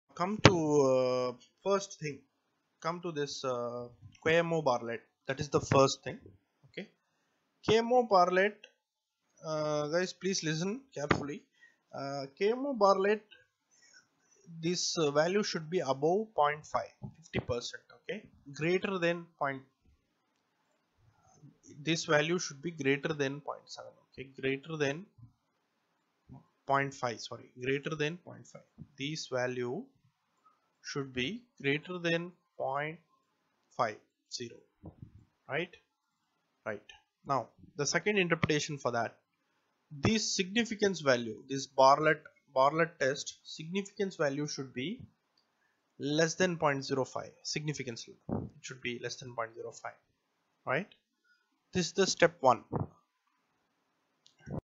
Exploratory Factor Analysis: Step 1/5 - KMO & Barlett Test (Business Analytics for MBA/PGDM)
This video is to understand the Exploratory Factor Analysis: - KMO & Barlett Test using SPSS in a simple and easy way.The dataset for the exploratory factor analysis can be downloaded from the